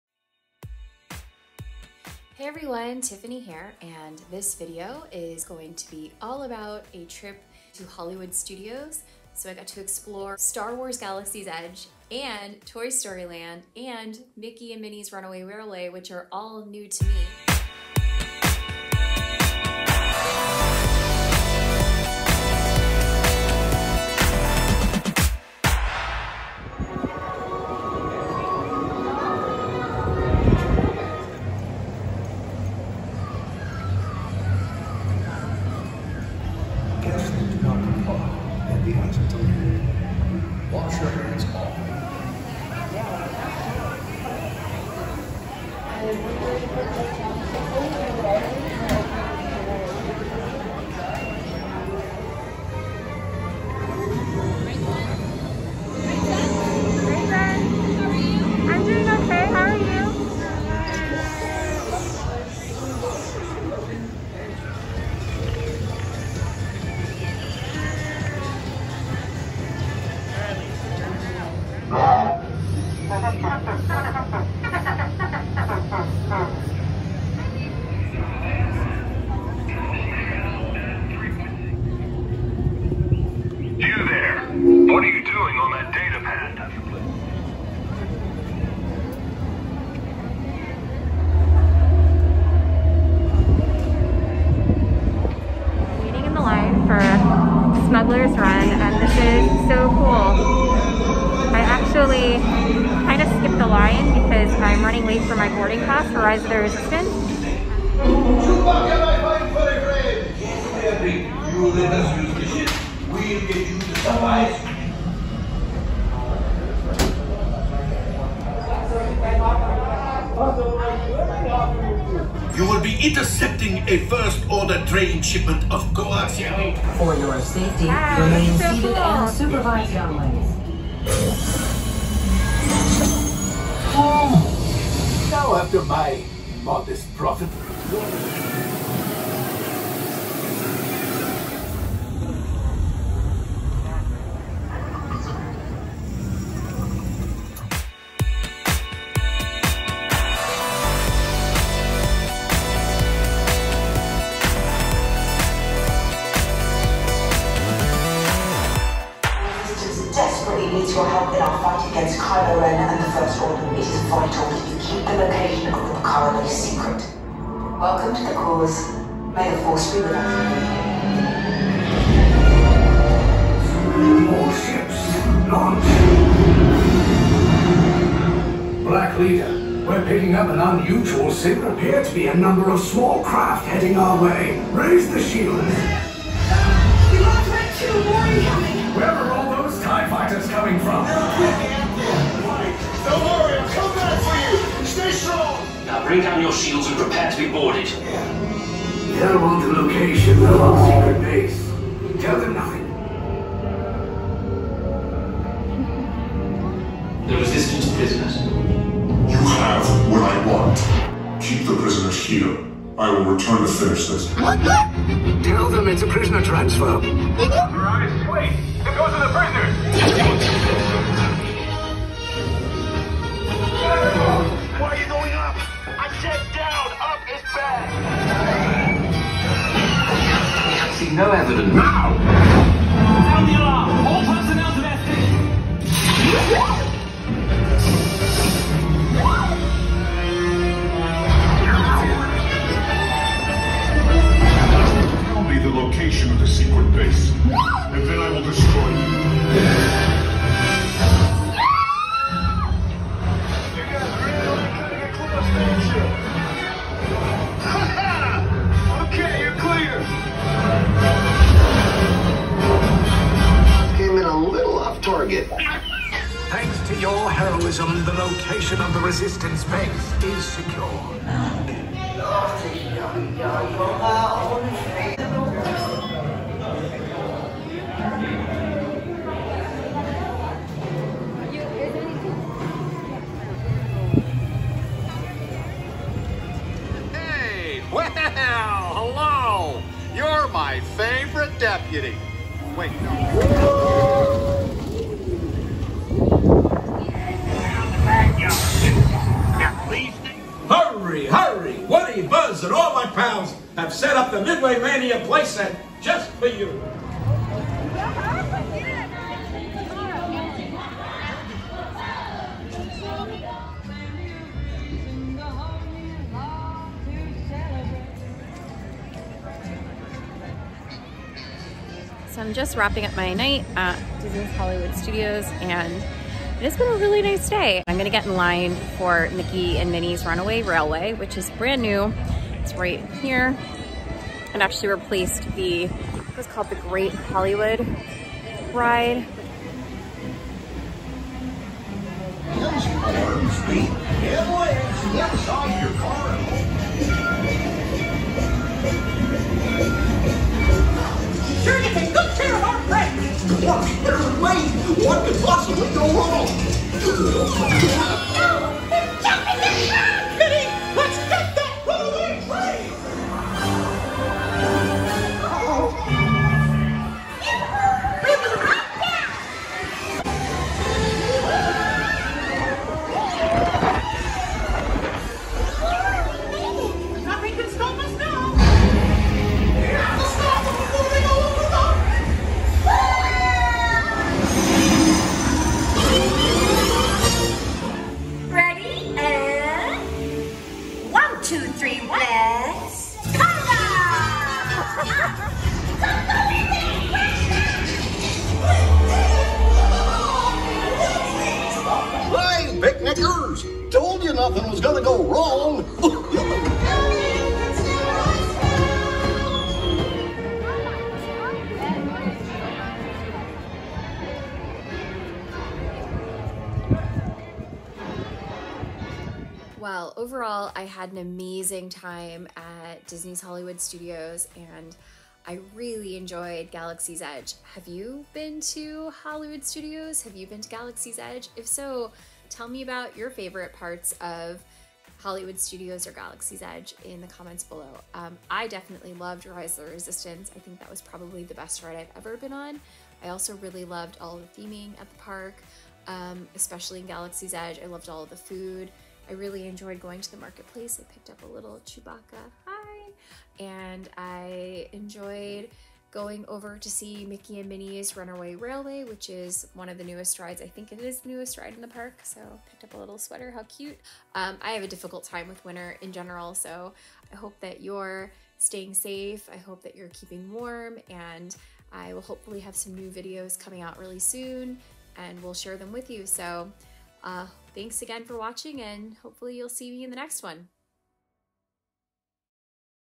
Galaxy’s Edge, Rise of the Resistance + Mickey and Minnie’s Runaway Railway| Disney Solo Trip
It’s been a while since I last uploaded, so Happy New Year. It’s a snowy day here in NYC, so let’s get back in the Winter spirit with a throwback trip to Disney’s Hollywood Studios.

I went on a solo Disney trip back in 2020, and caught some Christmas holiday decorations. I also visited Star Wars Galaxy’s Edge and Toy Story land for the first time! In Galaxy’s Edge I rode Rise of the Resistance, Millennium Falcon Smuggler’s Run, and I tried a Ronto Wrap, Braised Shaka Roast, and bought a Chewbacca doll in the market! In Toy Story land I loved Slinky Dog Dash and Alien Swirling Saucers. I especially enjoyed the newest ride Mickey and Minnie’s Runaway Railway.

The social distancing efforts were in place, and it felt safe. The lines were long, but manageable. I was able to do so much in just one day. Overall it was an amazing day.